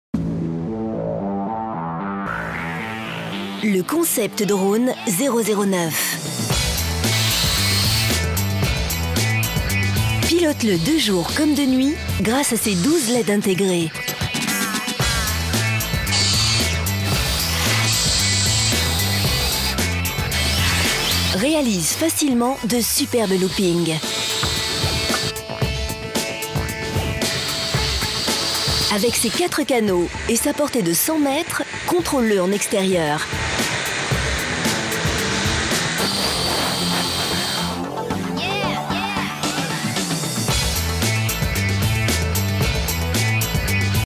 Concept Drone 009 AC-Déco
Ultra léger (2,4 g)
Longue portée : 100 mètres.
Lumière LED.
Chargeur USB.
Gyroscope.
Fonctions : haut / bas, avance / recule, gauche / droite, tourne sur lui-même, exécute des loopings